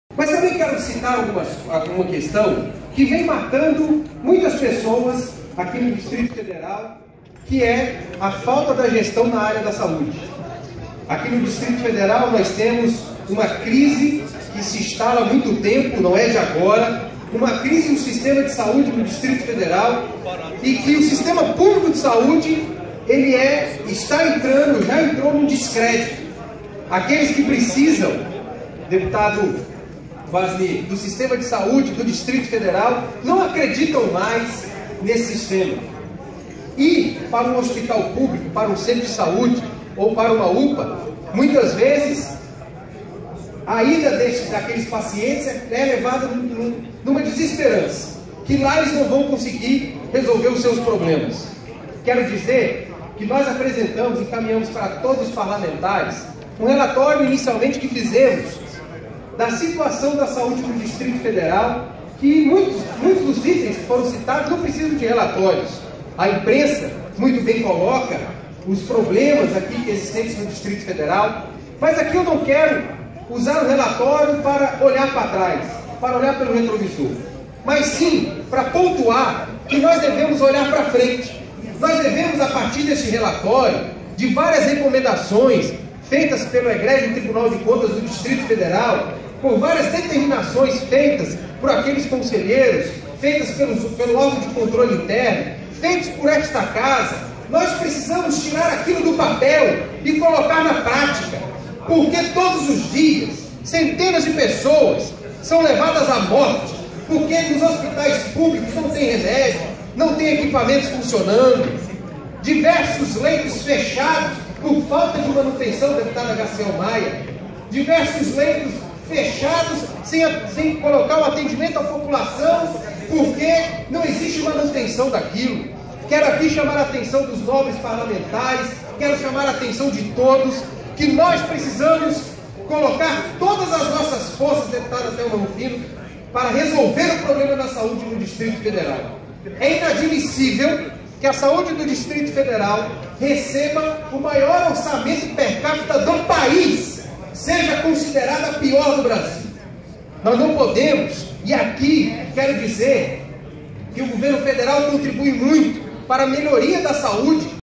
Plenário - Delmasso fala sobre a crise da saúde no DF 18/03/2015
Brasília enfrenta uma crise na saúde e já entrou em descrédito. Todos os dias centenas de pessoas são levadas à morte, pois os hospitais não tem medicamentos e insumos.